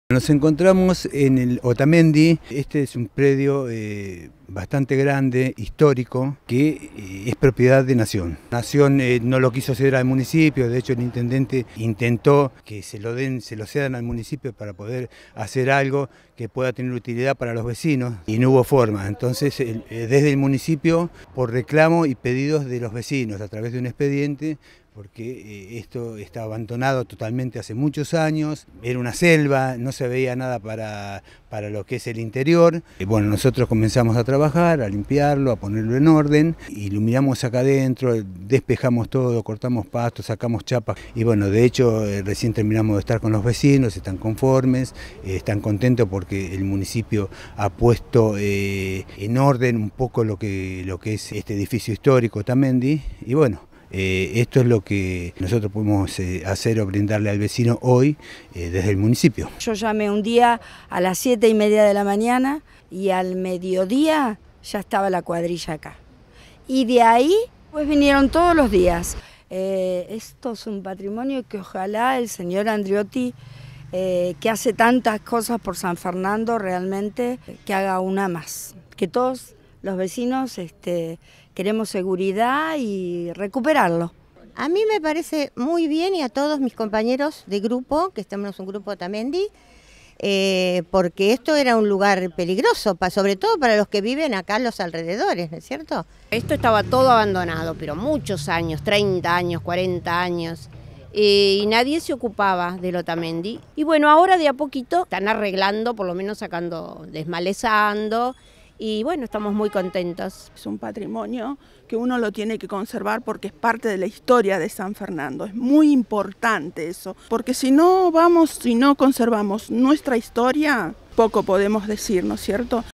Operativo de limpieza, poda y refuerzo de la seguridad del Palacio Otamendi
El histórico predio ubicado en Lavalle y Sarmiento, pertenece a la órbita nacional pero se encuentra abandonado y con custodia retirada. El Municipio pidió intervención a Nación para que brinde una solución, mientras tanto llevó adelante tareas de limpieza general y ordenamiento a pedido de los vecinos, presentes en el operativo.